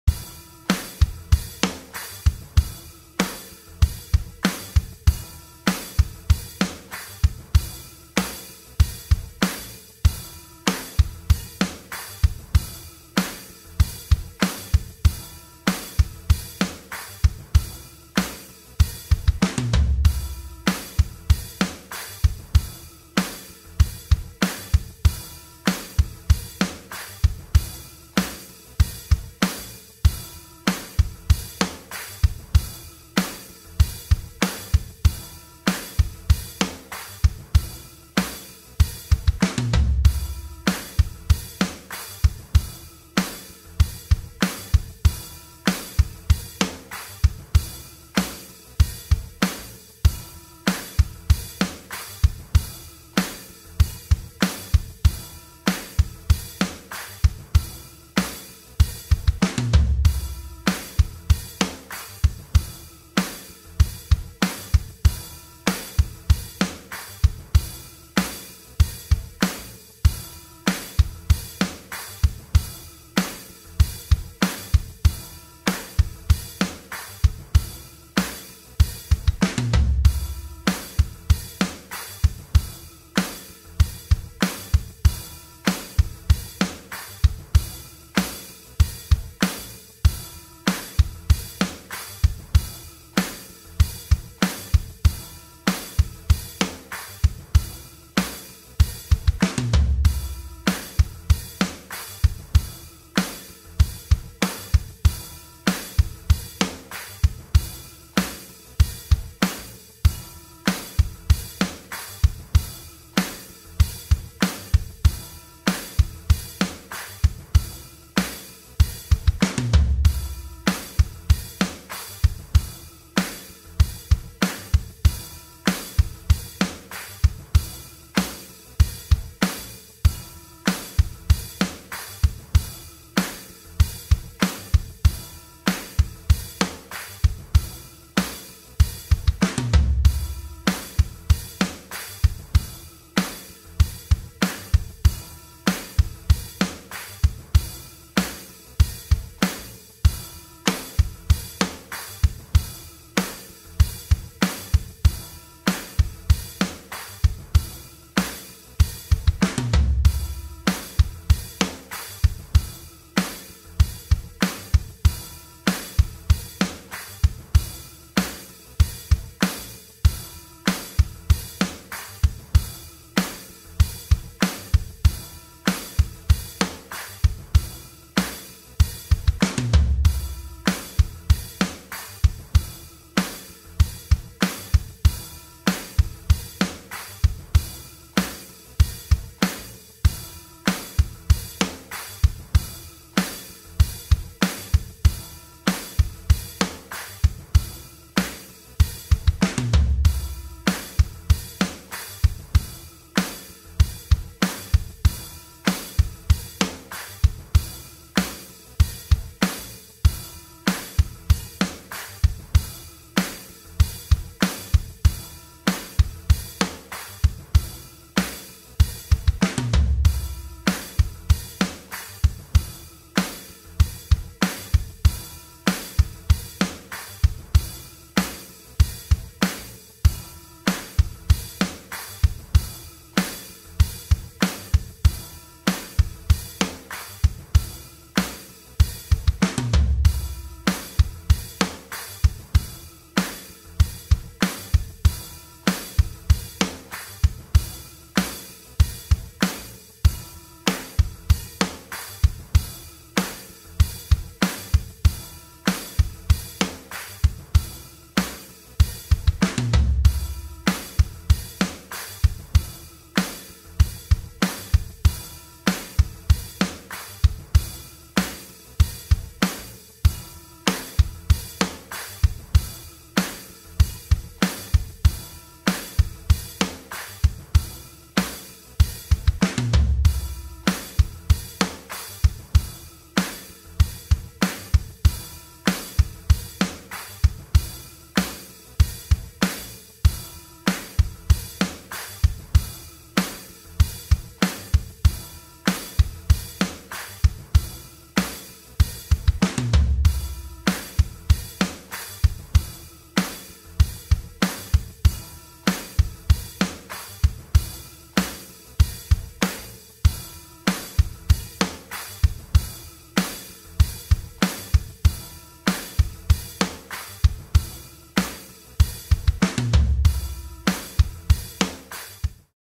Rock Hard Riffs Backing
Backing til de forskellige Rock Hard Riffs.
Sæt denne video på og øve til den. HUSK at den kan sætte ned i tempo her på youtube